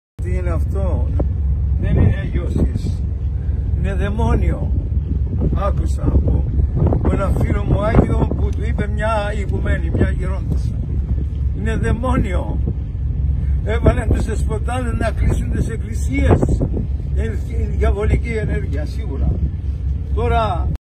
Γέροντας Σιλουανός ο Αθωνίτης : Είναι δαιμόνιο, _ Έβαλε τούς Δεσποτάδες να κλείσουν τις Εκκλησίες!
Έβαλε τούς Δεσποτάδες να κλείσουν τις Εκκλησίες; Είναι δαιμόνιο. Διαβολική ενέργεια... Σίγουρα. ~ π. Σιλουανός ο Αγιορείτης (Μαρτυρία σε προσκυνητή για το κλείσιμο των εκκλησιών). Απο τις πιο εμβληματικές και χαριτωμένες μορφές του Αγίου Όρους, γνωστός στούς τακτικούς επισκέπτες του Άθωνα...